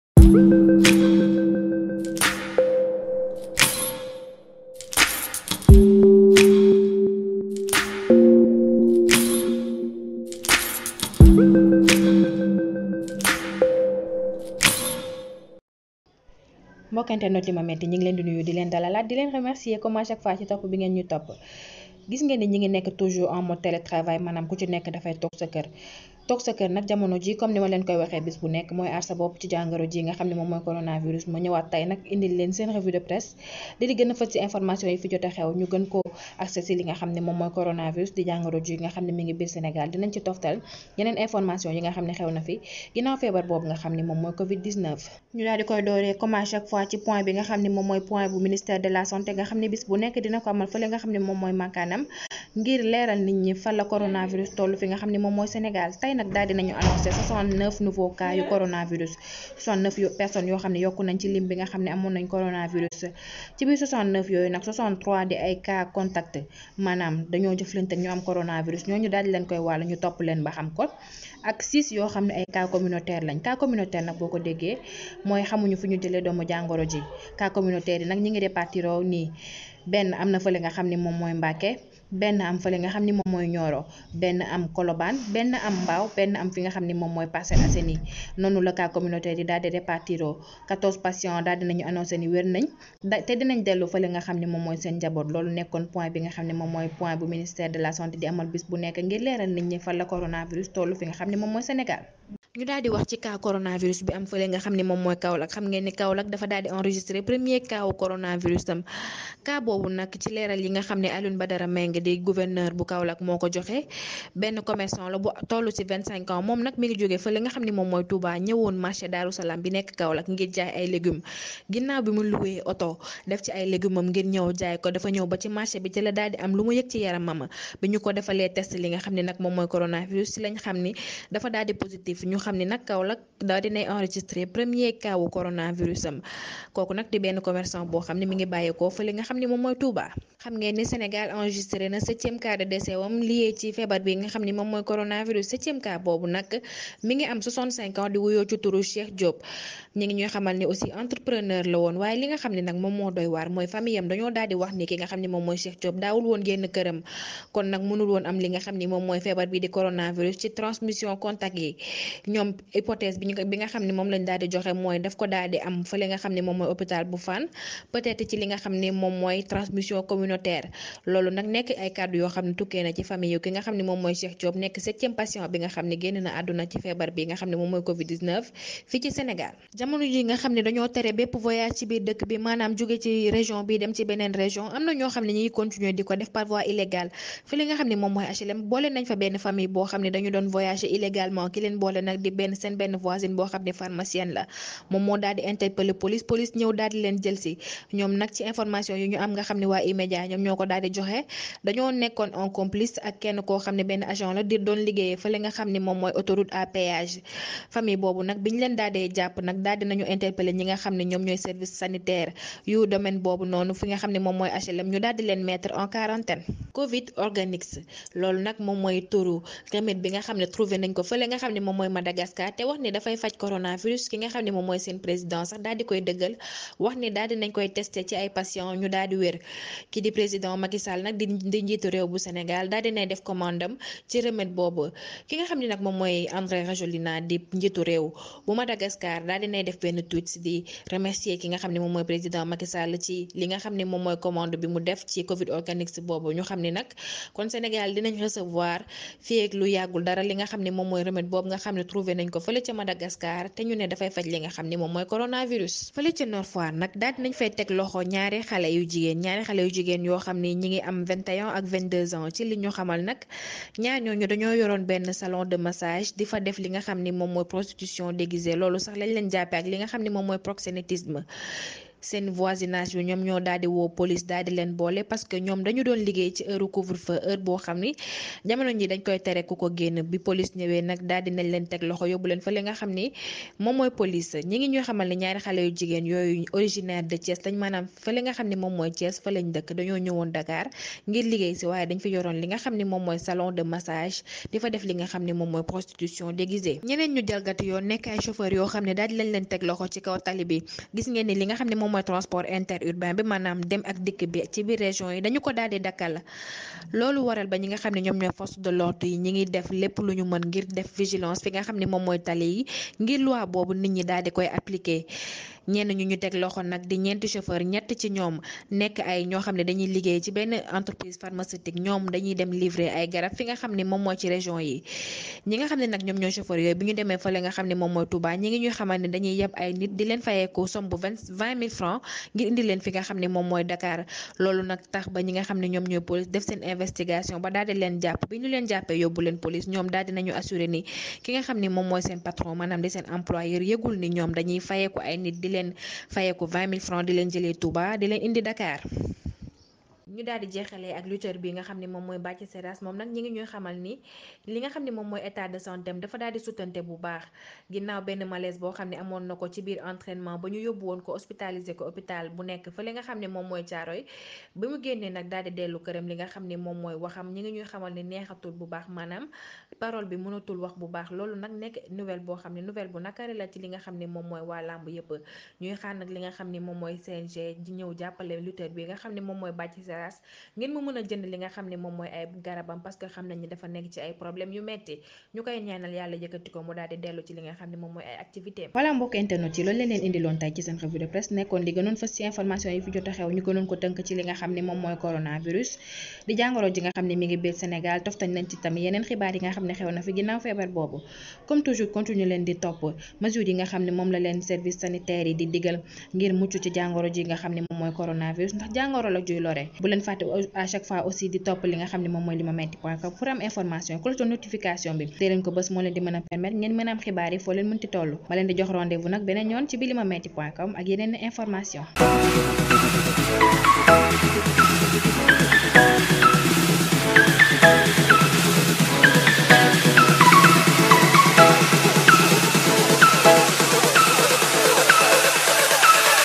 "Fèbar Bou Bone Bi", remède c0r0, pr0xénétisme, Bathie Séras... Tout ce que vous devez savoir !
Nous débutons avec, toujours cette pandémie de Coronavirus qui gagne du terrain sur le territoire sénégalais et le remède du Madagascar. Ensuite, un retour dans l'affaire de la famille aux HLM, avant de nous attaquer à l'affaire des deux masseuses de Ouest-foire. Et enfin, nous terminons avec le lutteur Bathie Séras qui se trouve dans un état critique.